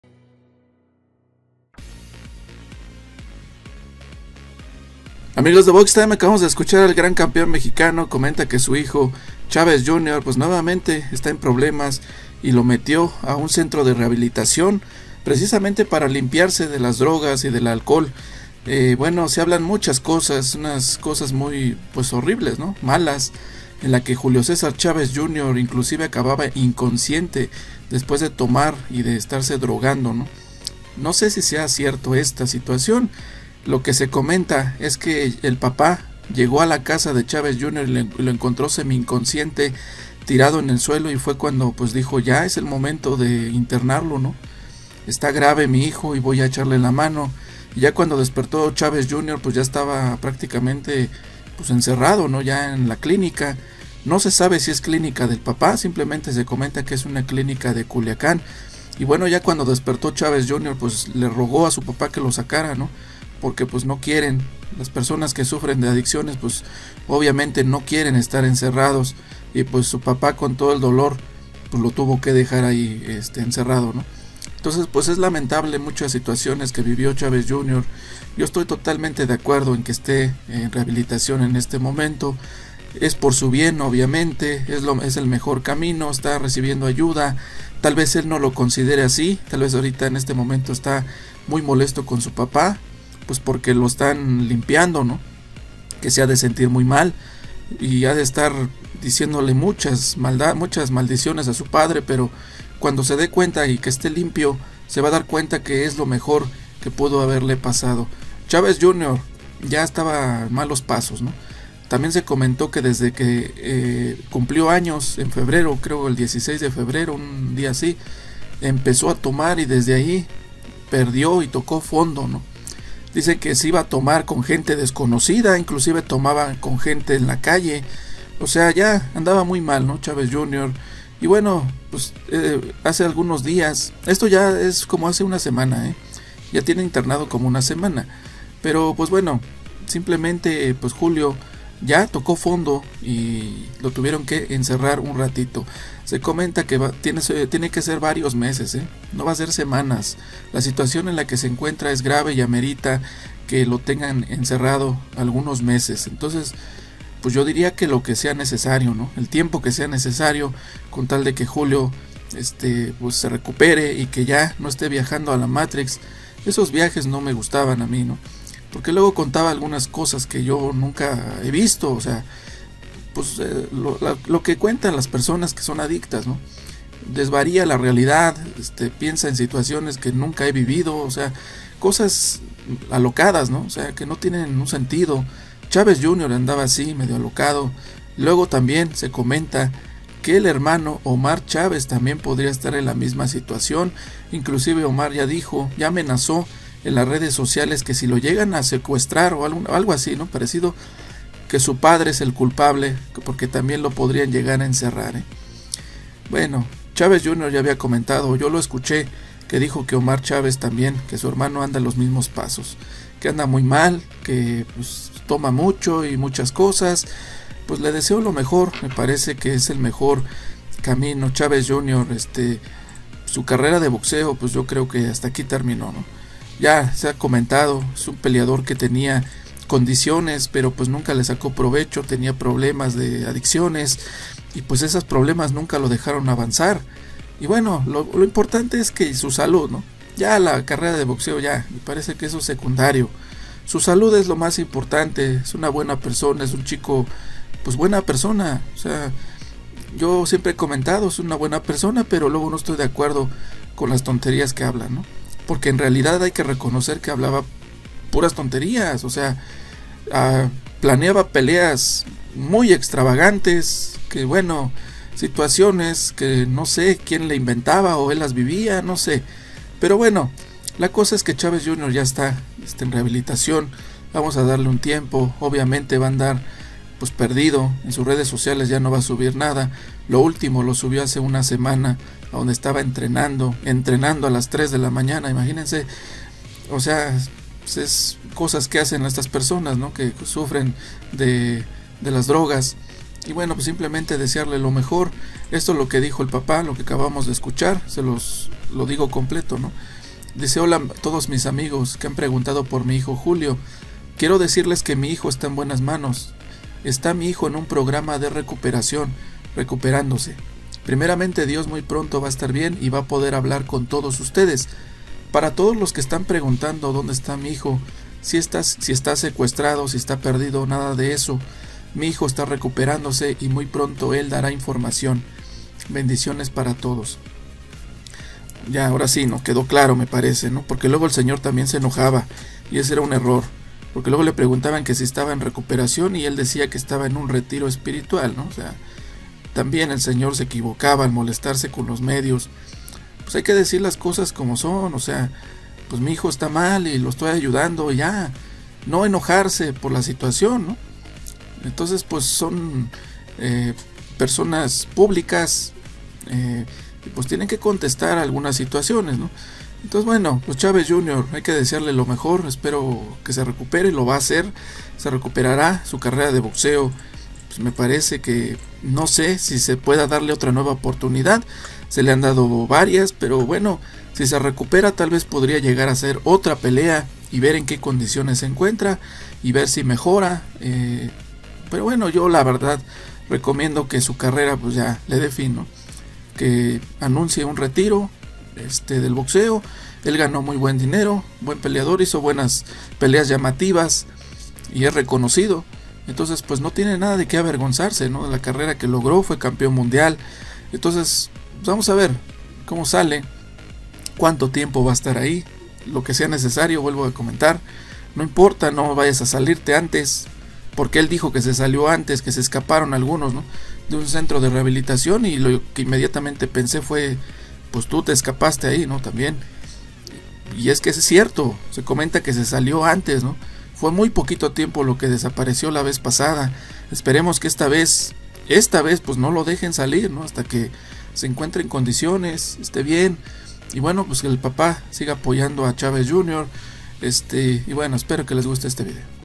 CHAVEZ JR TOCÓ FONDO Y ES INTERNADO NUEVAMENTE POR SU PAPA, OMAR TAMBIEN PODRÍA SER INTERVENIDO
El peleador Chavez Jr se encuentra internado por sus adicciones, su padre lo apoya y lo da a conocer.